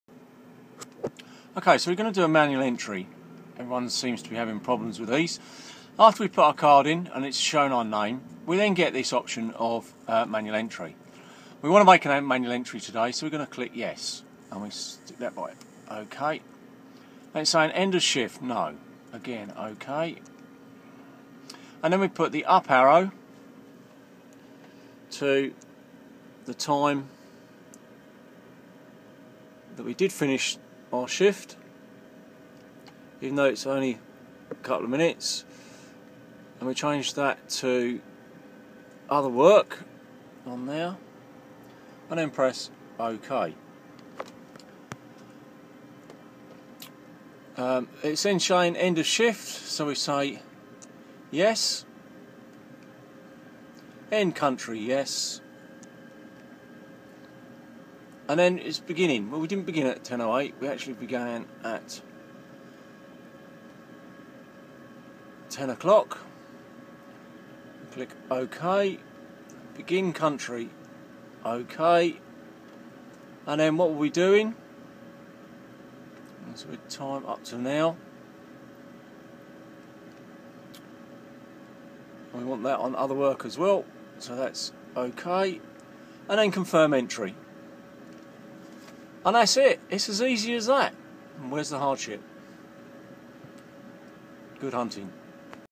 Digital tacho manual entry
How to do a manual entry on a digital tacho